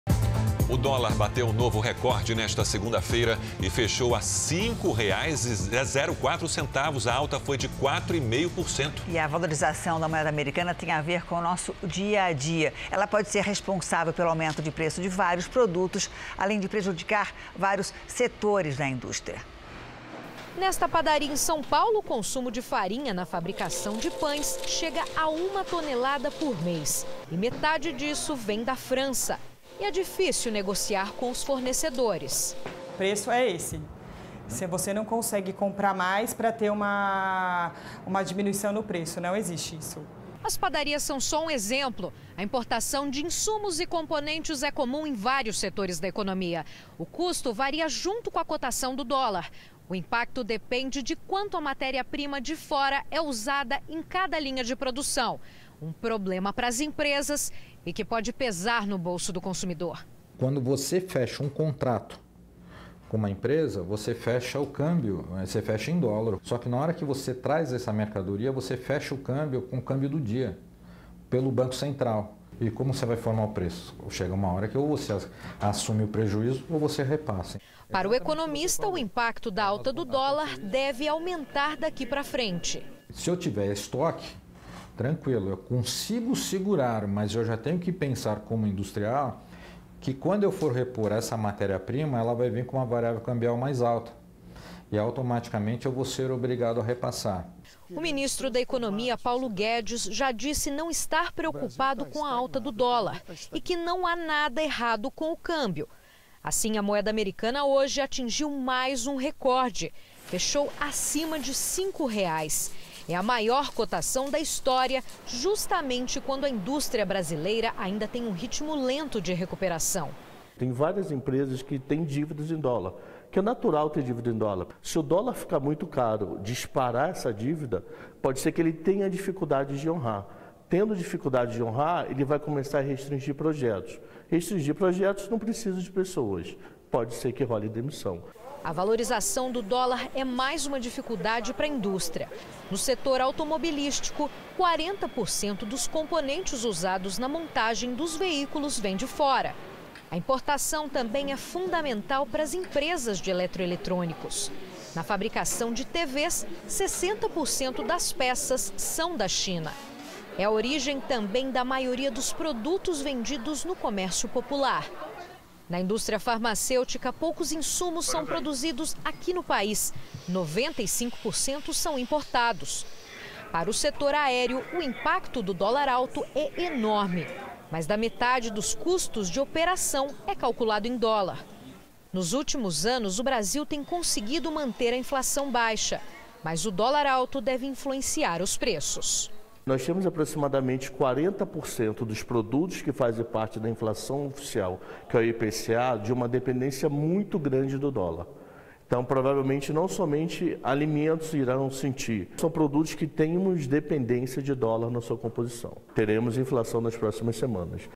Dólar bate novo recorde e fecha a segunda-feira (16) valendo R$ 5,04
A moeda americana teve alta de 4,5% e alcançou a maior cotação da história, valendo R$ 5,04. A valorização do dólar influência o aumento de preço de vários produtos, além de prejudicar diversos setores da indústria, que ainda tem um ritmo lento de recuperação.